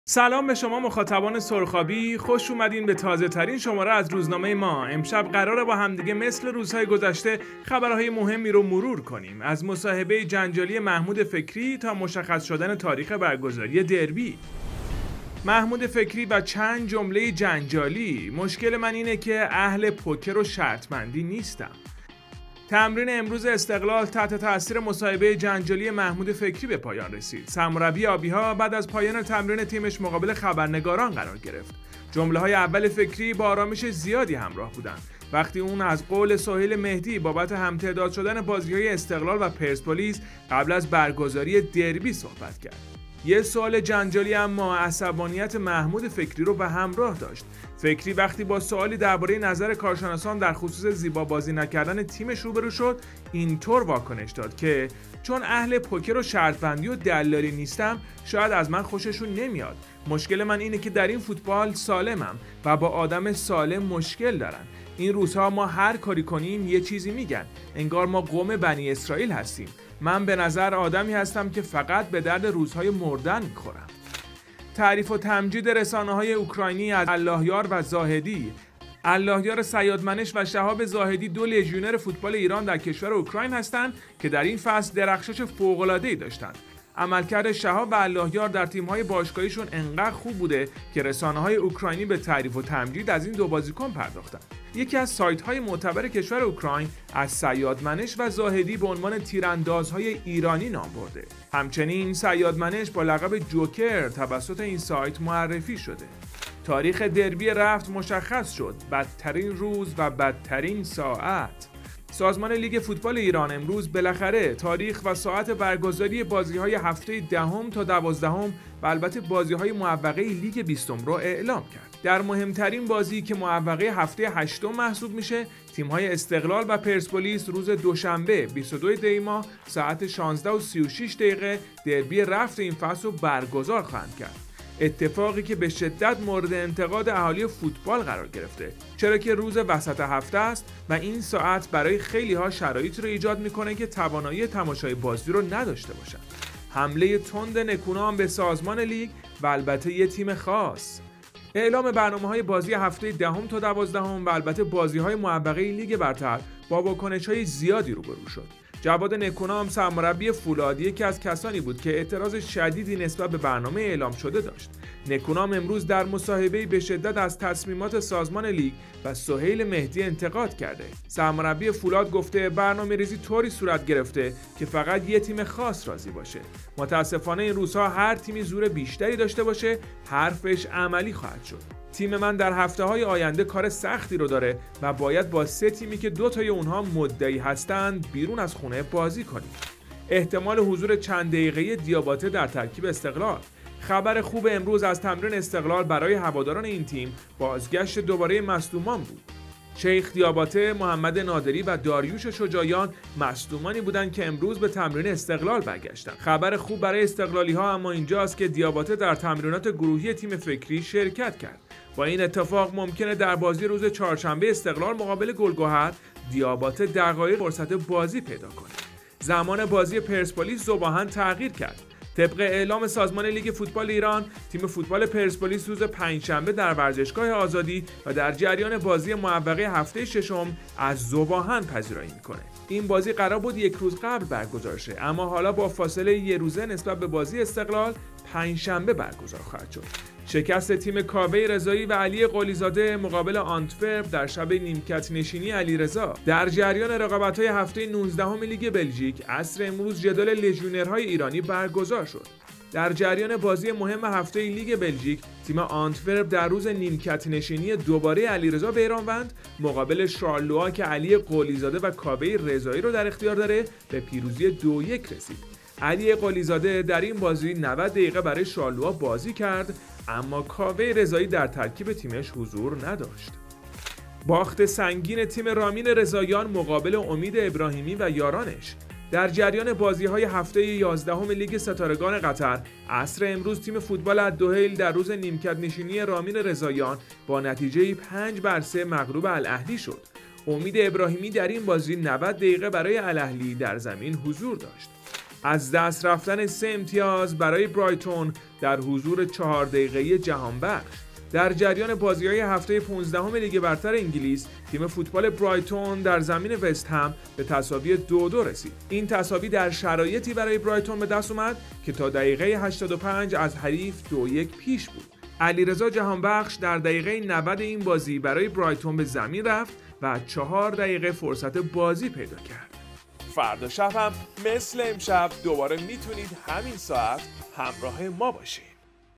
محمود فکری از کوره در رفت - تاریخ دربی مشخص شد، بدترین روز و بدترین ساعت - روزنامه سرخابی
سلام به شما مخاطبان سرخابی؛ خوش اومدین به تازه‌ترین شماره از روزنامه ما. امشب قراره با همدیگه مثل روزهای گذشته خبرهای مهمی رو مرور کنیم. از مصاحبه جنجالی محمود فکری تا مشخص شدن تاریخ برگزاری دربی.

محمود فکری و چند جمله جنجالی؛ مشکل من این است که اهل پوکر و شرط‌بندی نیستم!

تمرین امروز استقلال تحت‌تاثیر مصاحبه جنجالی محمود فکری به پایان رسید. سرمربی آبی‌ها بعد از پایان تمرین تیمش مقابل خبرنگاران قرار گرفت. جمله‌های اول فکری با آرامش زیادی همراه بودند. وقتی او از قول سهیل مهدی بابت هم‌تعداد شدن بازی‌های استقلال و پرسپولیس قبل از برگزاری دربی صحبت کرد. یک سوال جنجالی اما عصبانیت محمود فکری را به همراه داشت. فکری وقتی با سوالی درباره نظر کارشناسان در خصوص زیبا بازی نکردن تیمش روبرو شد؛ اینطور واکنش نشان داد که " چون اهل پوکر و شرط‌بندی و دلالی نیستم شاید از من خوش‌شان نمی‌آید. مشکل من این است که در این فوتبال سالم هستم و با آدم سالم مشکل دارند. این روزها ما هرکاری بکنیم یک چیز می‌گویند؛ انگار ما قوم بنی‌اسرائیل هستیم. من به نظر آدمی هستم که فقط به درد روزهای مردن می‌خورم!

تعریف و تمجید رسانه‌های اکراینی از اللهیار و زاهدی

اللهیار صیادمنش و شهاب زاهدی دو لژیونر فوتبال ایران در کشور اکراین هستند که در این فصل درخشش فوق‌العاده‌ای داشتند. عملکرد شهاب و اللهیار در تیم‌های باشگاهی‌شان انقدر خوب بوده که رسانه‌های اکراینی به تعریف و تمجید از این دو بازیکن پرداختند. یکی از سایت‌های معتبر کشور اکراین از صیادمنش و زاهدی به عنوان تیراندازهای ایرانی نام برده. همچنین صیادمنش با لقب جوکر توسط این سایت معرفی شده.

تاریخ دربی رفت مشخص شد؛ بدترین روز و بدترین ساعت!

سازمان لیگ فوتبال ایران امروز بالاخره تاریخ و ساعت برگزاری بازی‌های هفته دهم تا دوازدهم و البته بازی‌های معوقه لیگ بیستم را اعلام کرد. در مهم‌ترین بازی که معوقه هفته هشتم محسوب می‌شود تیم‌های استقلال و پرسپولیس روز دوشنبه 22 دی ماه ساعت 16:36 دربی رفت این فصل را برگزار خواهند کرد. اتفاقی که به شدت مورد انتقاد اهالی فوتبال قرار گرفته. چراکه روز وسط هفته و این ساعت برای خیلی‌ها شرایطی را ایجاد می‌کند که توانایی تماشای بازی را نداشته باشند.

حمله تند نکونام به سازمان لیگ و البته یک تیم خاص!

اعلام برنامه بازی‌های هفته دهم تا دوازدهم و البته بازی‌های معوقه لیگ برتر با واکنش‌های زیادی روبرو شد. جواد نکونام سرمربی فولاد یکی از کسانی بود که اعتراض شدیدی نسبت به برنامه اعلام شده داشت. نکونام امروز در مصاحبه‌ای به شدت از تصمیمات سازمان لیگ و سهیل مهدی انتقاد کرده. سرمربی فولاد گفته برنامه‌ریزی طوری صورت گرفته که فقط یک تیم خاص راضی باشد. متاسفانه این روزها هر تیمی زور بیشتری داشته باشد، حرفش عملی خواهد شد. تیم من در هفته‌های آینده کار سختی دارد و باید با سه تیم که دوتای آنها مدعی هستند، بیرون از خانه بازی کنیم.

احتمال حضور چند دقیقه‌ای دیاباته در ترکیب استقلال

خبر خوب امروز از تمرین استقلال برای هواداران این تیم بازگشت دوباره مصدومان بود. شیخ دیاباته، محمد نادری و داریوش شجاعیان مصدومانی بودند که امروز به تمرین استقلال برگشتند. خبر خوب برای استقلالی‌ها اما اینجاست که دیاباته در تمرینات گروهی تیم فکری شرکرت کرد. با این اتفاق ممکن است در بازی روز چهارشنبه استقلال مقابل گل‌گهر، دیاباته دقایقی فرصت بازی پیدا کند.

زمان بازی پرسپولیس - ذوب‌آهن تغییر کرد

طبق اعلام سازمان لیگ فوتبال ایران تیم فوتبال پرسپولیس روز پنجشنبه در ورزشگاه آزادی و در جریان بازی معوقه هفته ششم از ذوب‌آهن پذیرایی خواهد کرد. این بازی قرار بود یک روز قبل برگزار شود اما حالا با فاصله یک روزه نسبت به بازی استقلال، پنجشنبه برگزار خواهد شد.

شکست تیم کاوه رضایی و علی قلی‌زاده مقابل آنتورپ در شب نیمکت‌نشینی علیرضا

در جریان رقابت‌های نوزدهم لیگ بلژیک عصر امروز جدال لژیونرهای ایرانی برگزار شد. در جریان بازی مهم هفته لیگ بلژیک تیم آنتورپ در روز نیمکت‌نشینی دوباره علیرضا بیرانوند مقابل شارلوا که علی قلی‌زاده و کاوه رضایی را در اختیار دارد به پیروزی 2-1 رسید. علی قلی‌زاده در این بازی 90 دقیقه برای شارلوا بازی کرد اما کاوه رضایی در ترکیب تیمش حضور نداشت.

باخت سنگین تیم رامین رضاییان مقابل امید ابراهیمی و یارانش

در جریان بازی‌های هفته یازدهم لیگ ستارگان قطر عصر امروز تیم فوتبال الدحیل در روز نیمکت‌نشینی رامین رضاییان با نتیجه 5-3 مغلوب الاهلی شد. امید ابراهیمی در این بازی 90 دقیقه برای الاهلی در زمین حضور داشت.

از دست رفتن سه امتیاز برای برای برایتون در حضور چهار دقیقه‌ای جهانبخش

در جریان بازی‌های هفته پانزدهم لیگ برتر انگلیس تیم فوتبال برایتون در زمین وستهام به تساوی 2-2 رسید. این تساوی در شرایطی برای برایتون بدست آمد که تا دقیقه 85 از حریف 2-1 پیش بود. علیرضا جهانبخش در دقیقه 90 این بازی برای برایتون به زمین رفت و چهار دقیقه فرصت بازی پیدا کرد.

مارو حتما در اینستاگرام دنبال کنید 📺🔥
Instagram.com/Sorkhabii.TV
کانال تلگرامی سرخابی 🌏
Telegram.me/Sorkhabiitv